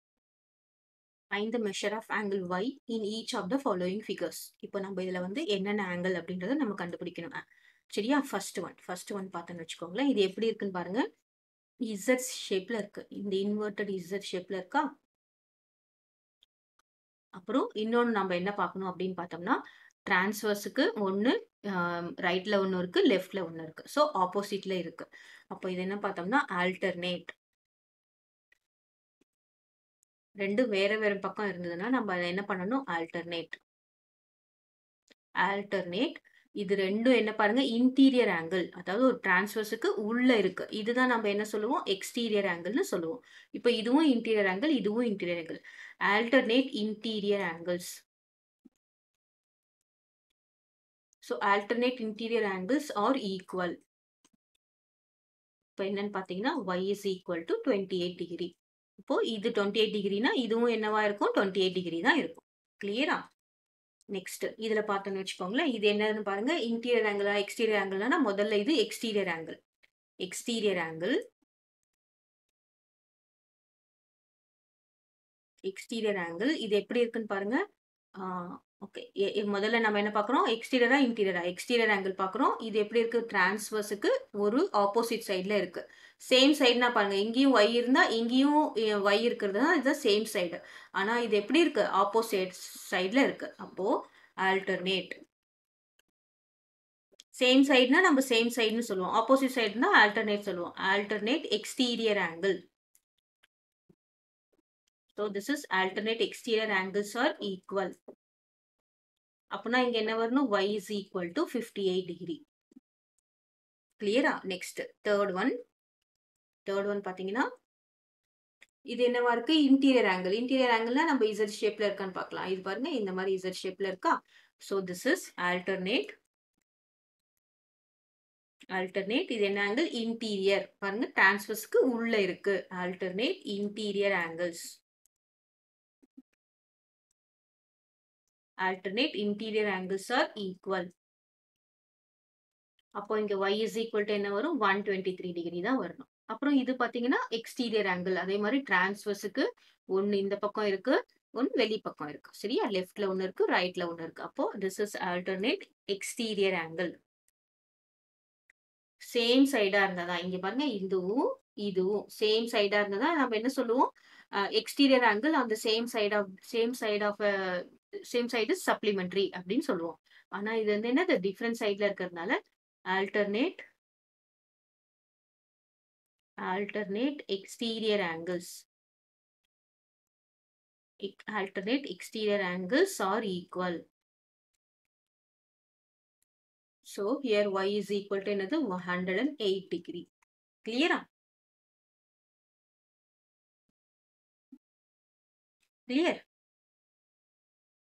Class 7 TN Maths Term I 5. Geometry Exercise 5.2 3. Find the measure of angle y in each of the
TN Term I  5. Geometry
Exercise 5.2
3. Find the measure of angle y in each of the following